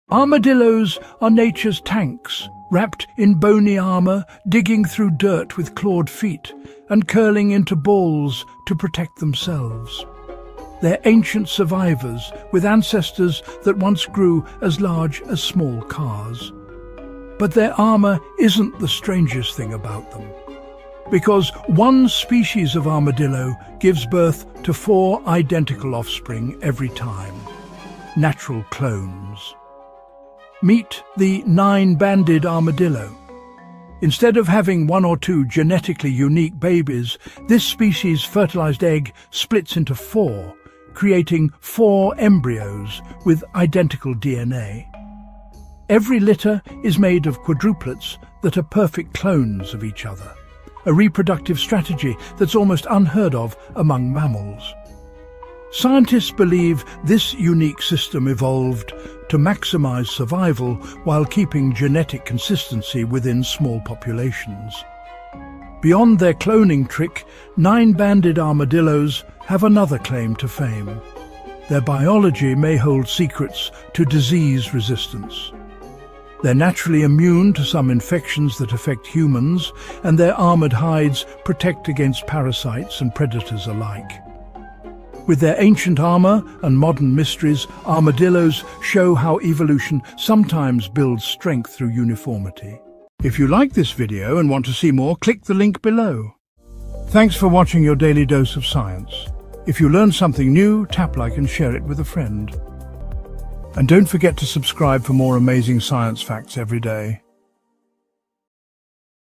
The 9 Banded Armadillo has 4 Identical Clones as Babies. Find out How This Happens.
The nine-banded armadillo isn’t just known for its armor — it’s one of the few mammals that naturally clones itself. Each time it gives birth, it produces four identical offspring, all sharing the exact same DNA. This biological wonder has fascinated scientists for decades, offering insight into genetics, reproduction, and evolution. Armadillos are also ancient survivors, descended from prehistoric giants that once roamed the Americas. With their tough shells, sharp claws, and uncanny ability to create identical quadruplets, armadillos are among nature’s most extraordinary mammals.